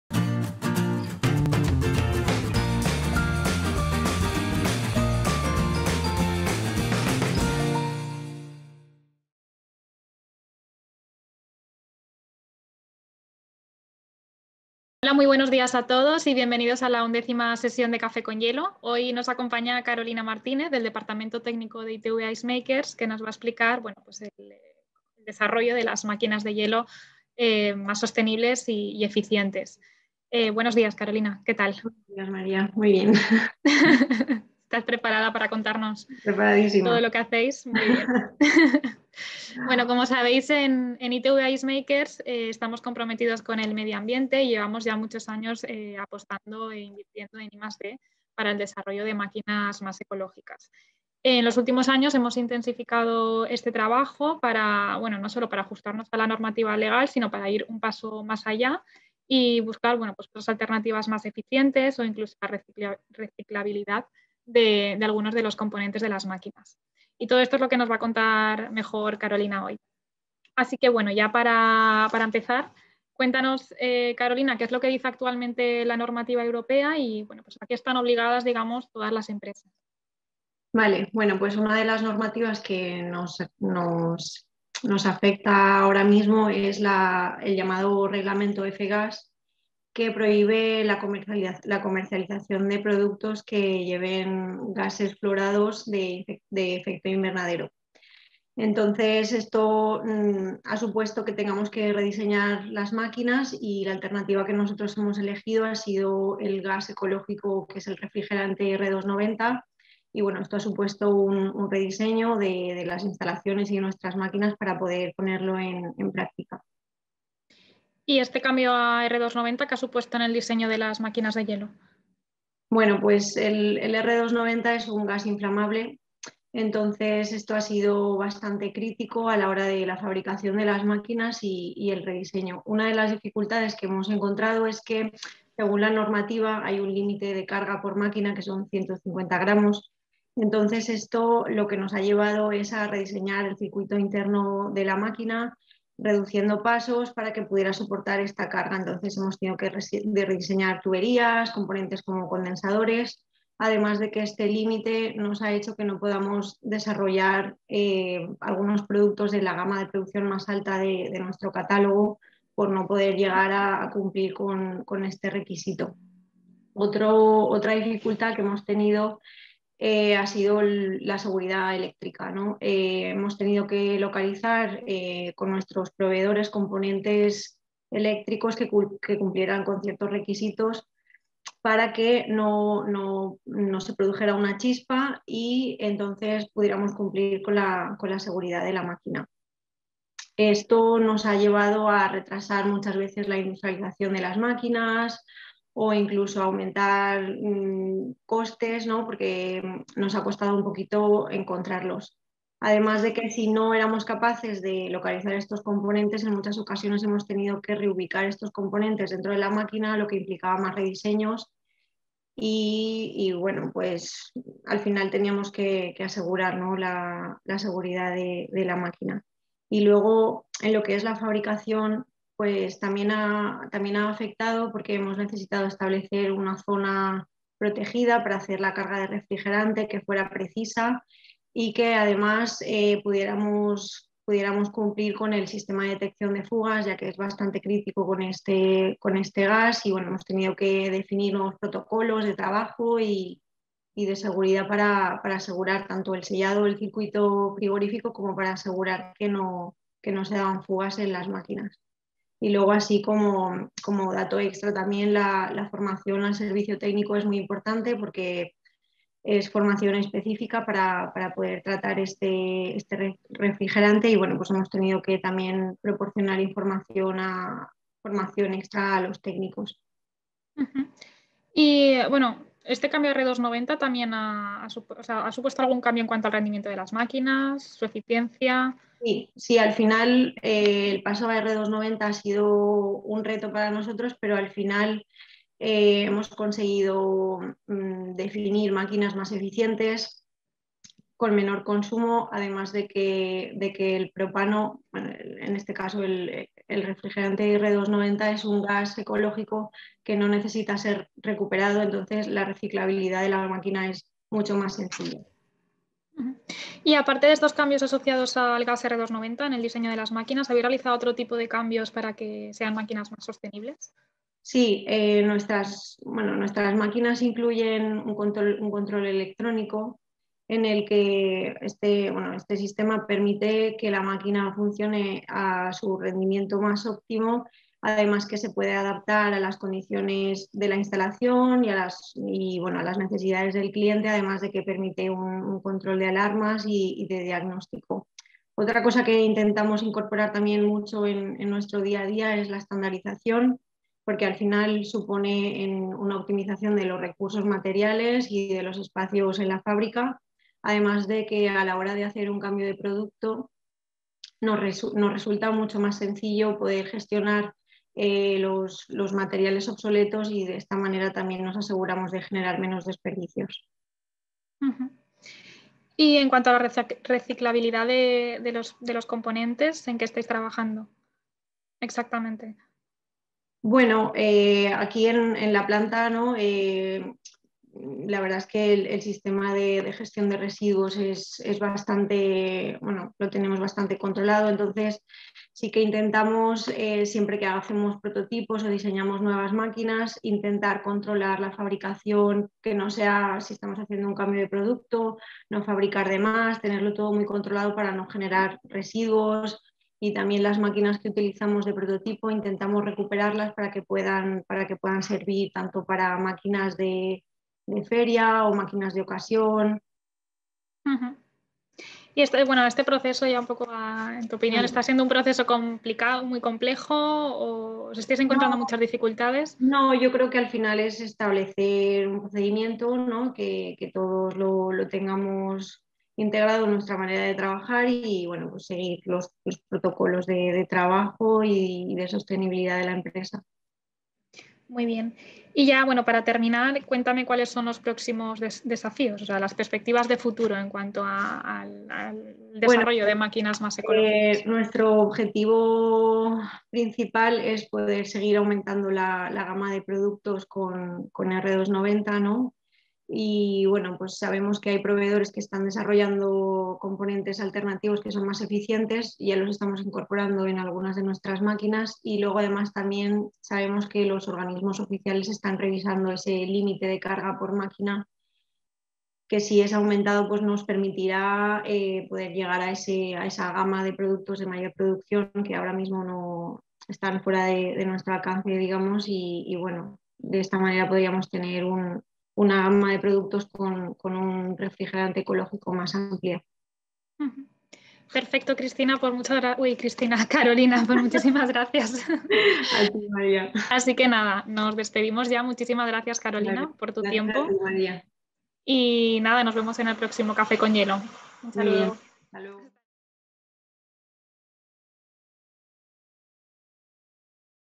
Café con hielo: Desarrollo de máquinas de hielo más ecológicas y eficientes. Retos y Desafíos.
Charla con Carolina Martínez, Departamento de Oficina Técnica de ITV Ice Makers